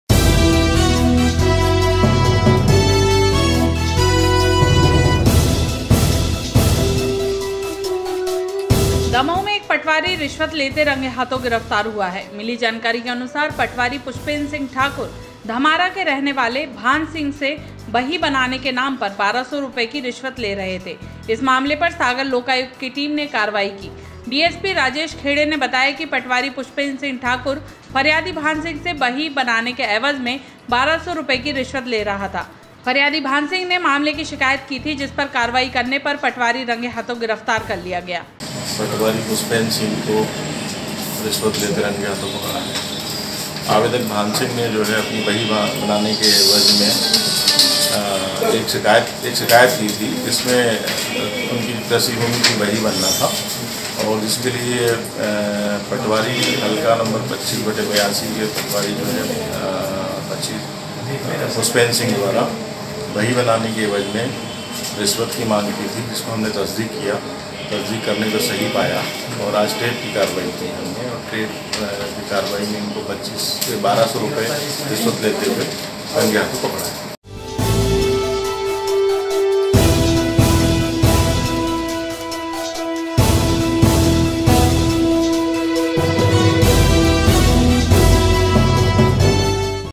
दमोह में पकड़ाया रिश्वतखोर पटवारी
दमोह में एक पटवारी 1200 रुपए की रिश्वत लेते रंगे हाथों गिरफ्तार हुआ है... पटवारी पुष्पेंद्र सिंह ठाकुर बही बनाने के एवज में रुपए ले रहा था...